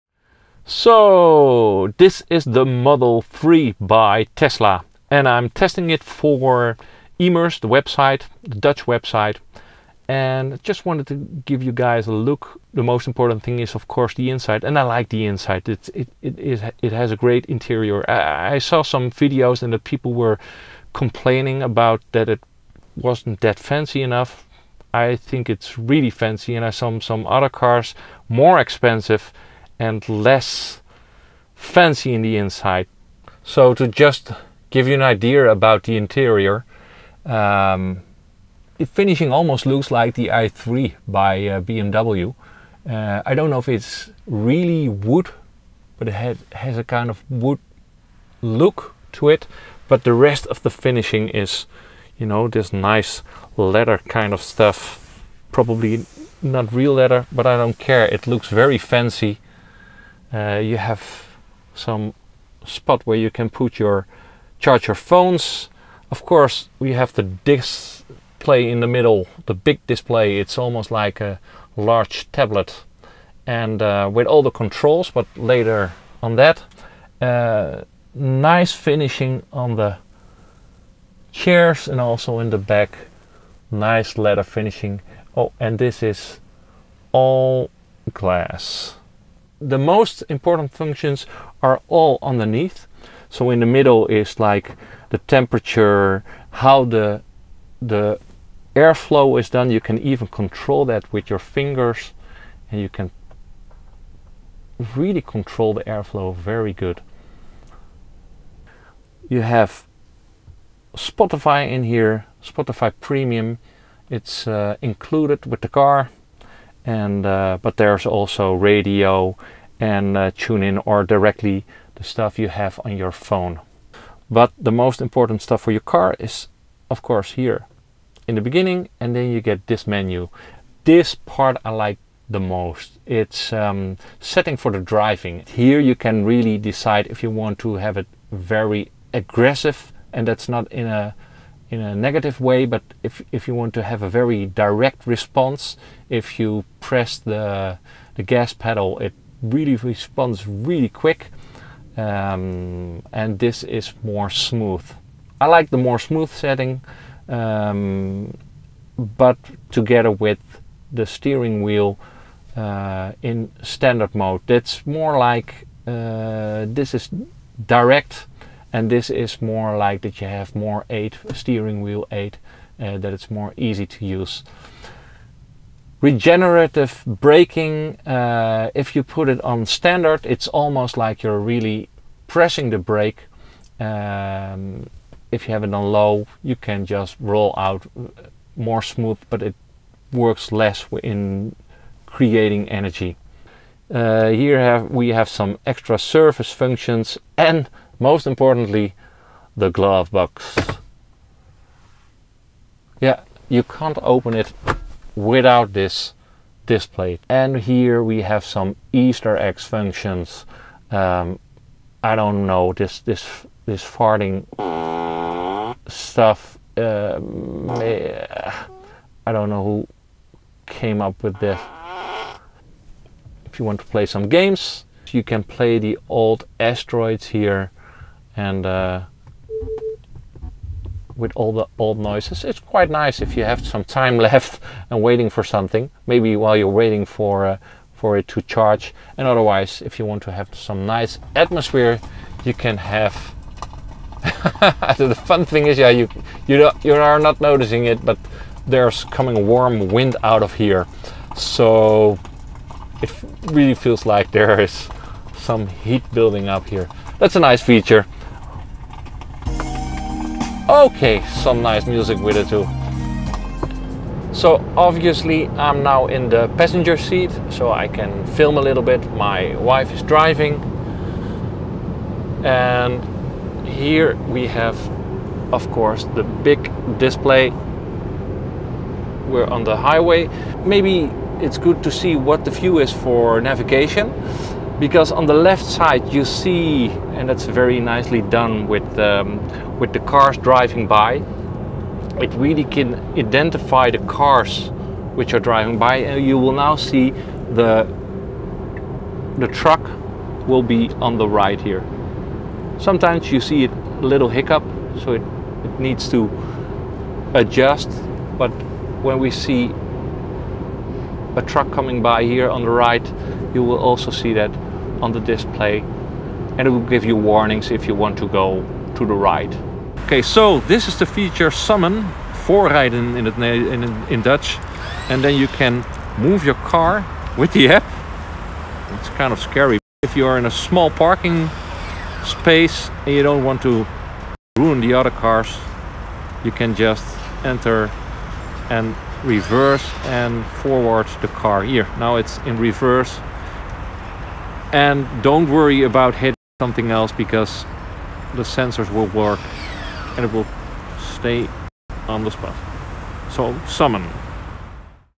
What I liked about the Tesla Model 3 after reviewing it
Here are the things I liked about the Tesla Model 3 after reviewing it for Dutch website Emerce.

0:00 Introduction
0:34 The interior
1:29 The display: airflow, Spotify, driving settings
3:14 Opening the glovebox
3:27 Easter Eggs: Fart Whoopie Cushion, Atari games, Fireplace
4:24 On the Highway, car detection
5:20 Tesla App Summon function